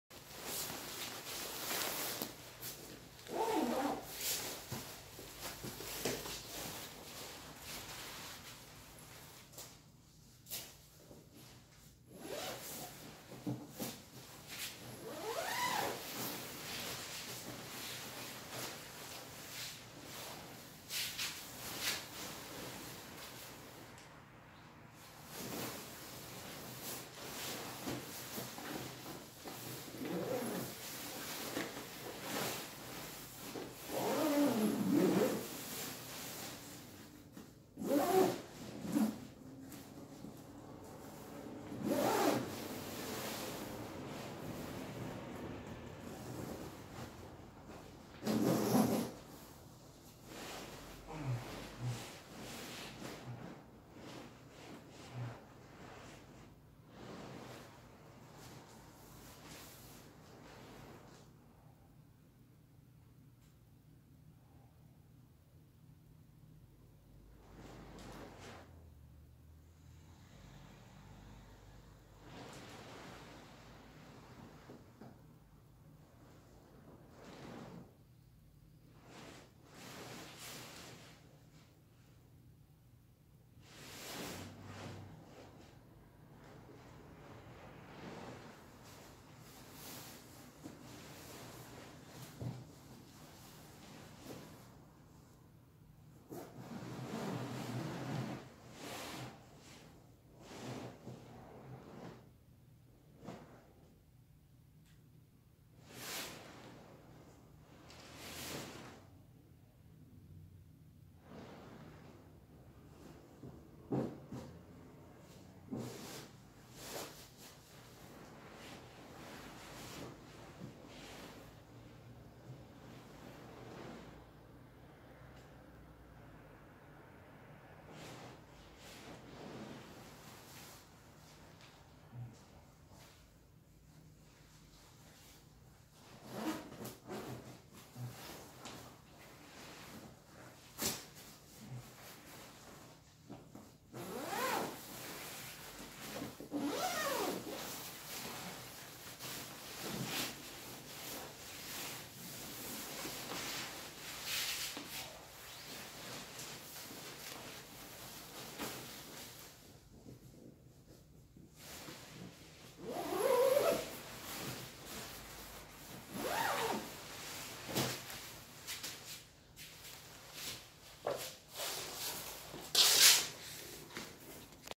western mountaineering puma gws backpacking sleeping bag / 웨스턴 마운티니어링 푸마GWS 백패킹 극동계 침낭 + 우모복 레이어링
웨스턴 마운티니어링 퓨마GWS.패킹 / western mountaineering puma gws Expedition 백패킹 극동계 침낭

wm ratings -32도
850fp+ 구스

상의 : 플리스 + 퍼머프로스트(250g)
하의 : 루베르 일반(180g)

- Sleeping bag size : L / 200cm
- 가슴폭 : 84cm / 실측 80cm
- 1135g / 1785g

저 173cm / 73kg

★사이즈.로프트.퀄리티를 보려고 구입했던 침낭이라
실사용 못해본 침낭들이 대부분이고
현재 몇개 없습니다. 참고만 해주세요~